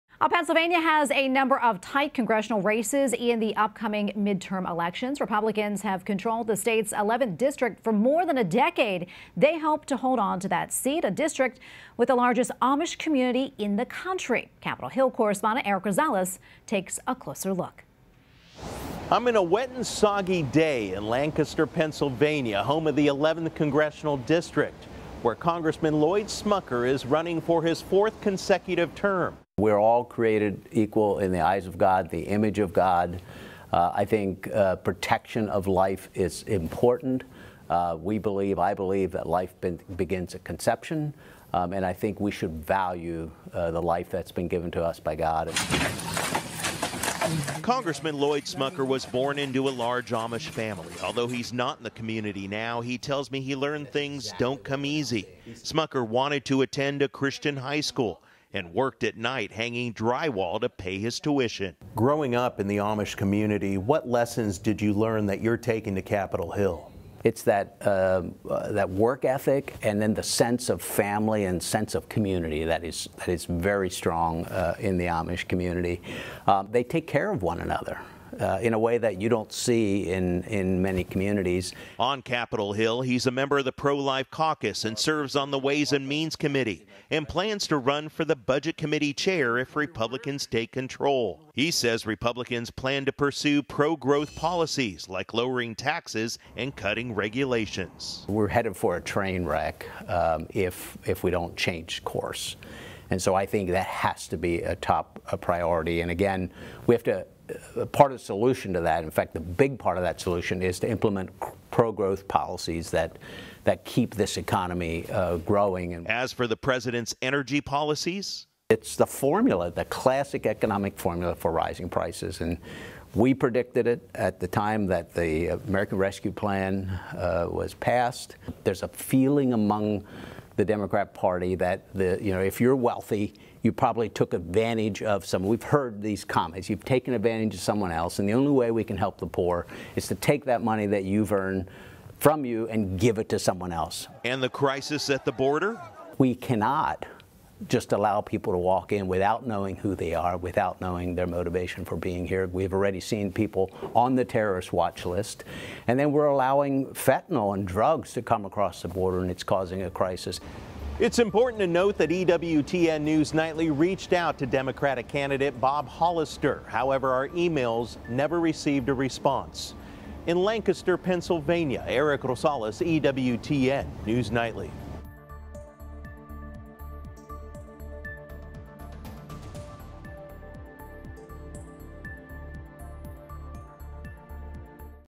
Republicans Hope to Hold onto District 11 in Pennsylvania with Lloyd Smucker | EWTN News Nightly
Pennsylvania has a number of tight Congressional races in the upcoming midterm elections. Republicans have controlled the state's 11th district for more than a decade. They hope to hold onto the seat in the district with the largest Amish community in the country. Congressman Lloyd Smucker is running for his 4th consecutive term in district 11. Smucker says, "We are all created equal in the eyes of God, the image of God. I think protection of life is important. I believe that life begins at conception and I think we ought to value the life that's been given to us by God." Congressman Smucker was born into a large Amish family and although he is not in the community now, he says he learned that things don't come easy. Smucker wanted to attend a Christian high school and worked at night hanging drywall to pay his tuition. On Capitol Hill, he's a member of the Pro-Life Caucus, serves on the Ways and Means Committee and plans to run for Budget Committee Chair if Republicans take control. Capitol Hill Correspondent, Erik Rosales reports.